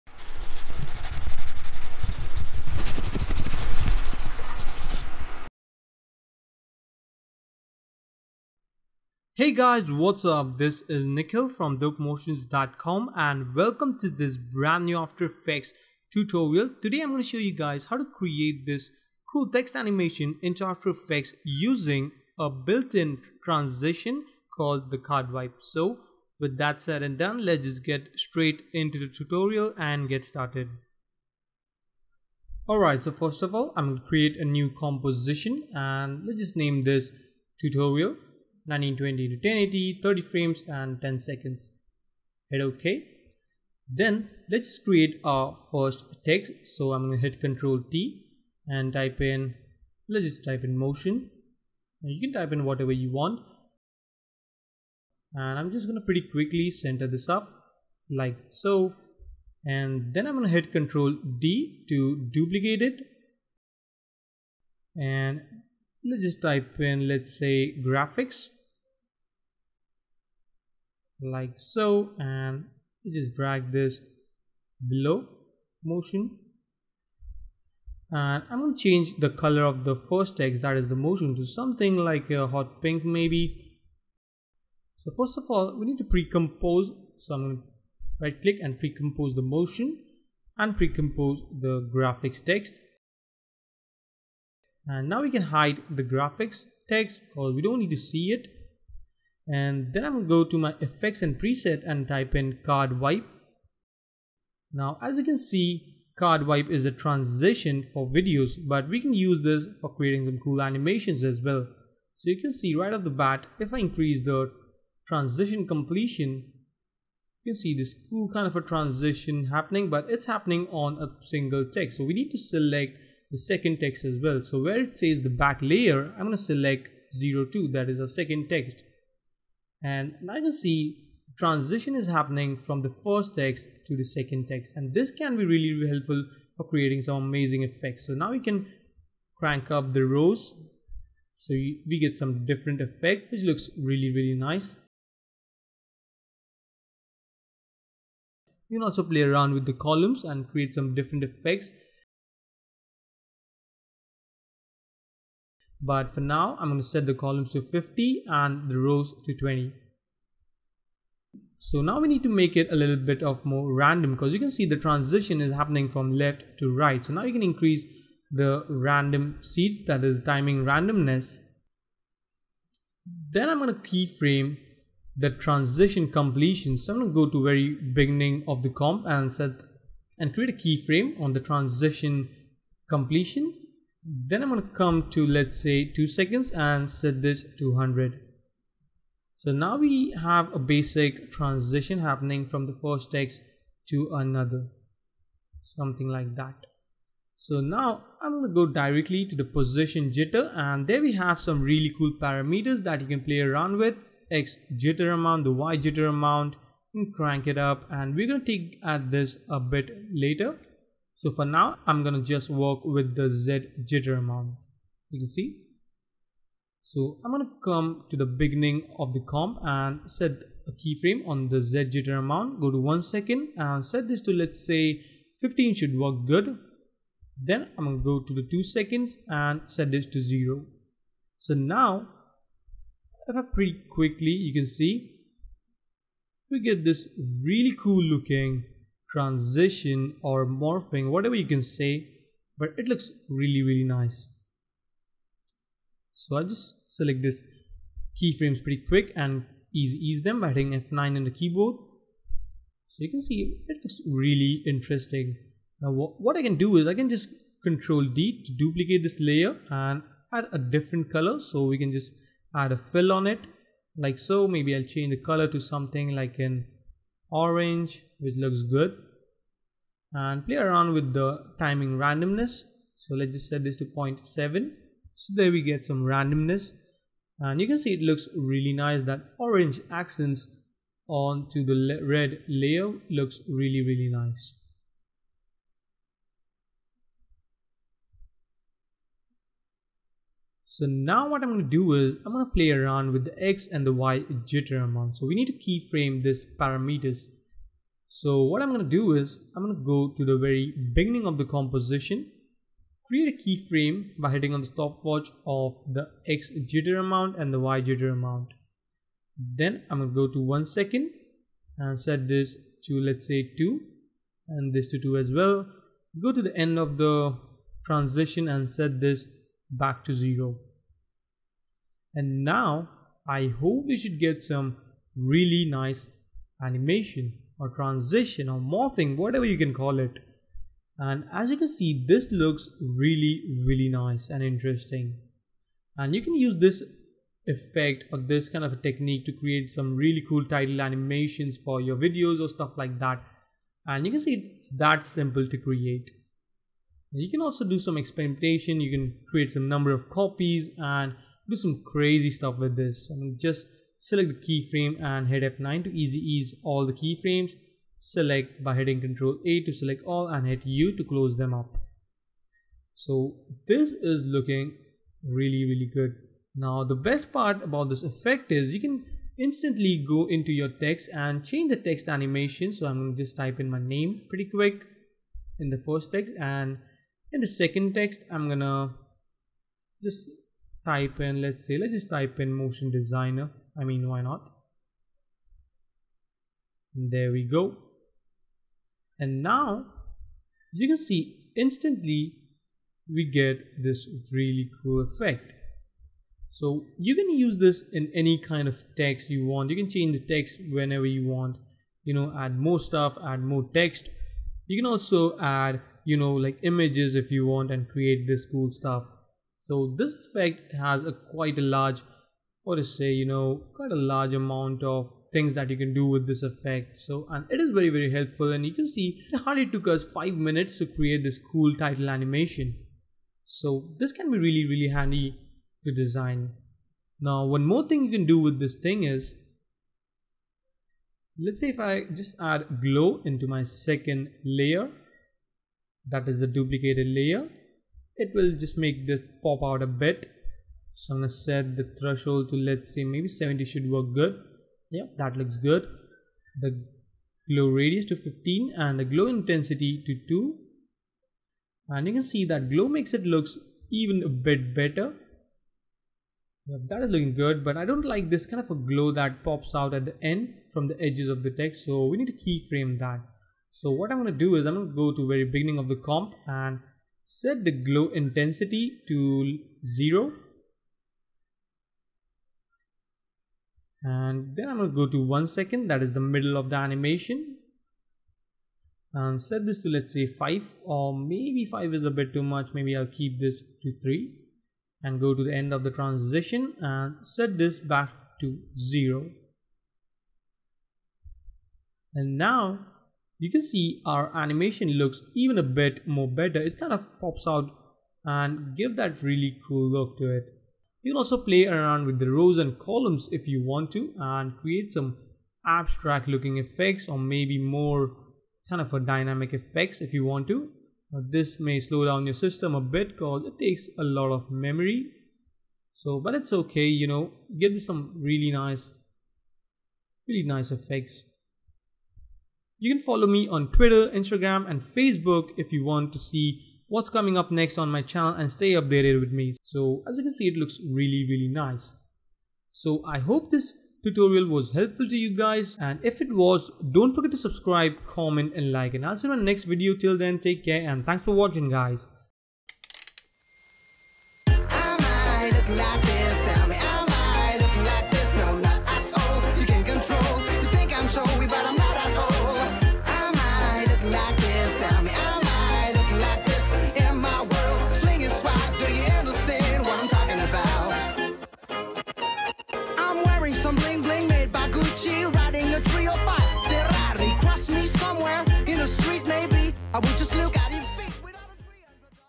After Effects Tutorial : Text Animation with Card Wipe
Heys guys wassup in this tutorial video we will be learning how to create cool and awesome looking text animation into after effects so its gonna be fun and i hope you will learn something new today !

Like my videos? Hate my videos? Let me know, I'm happy to help if you've got questions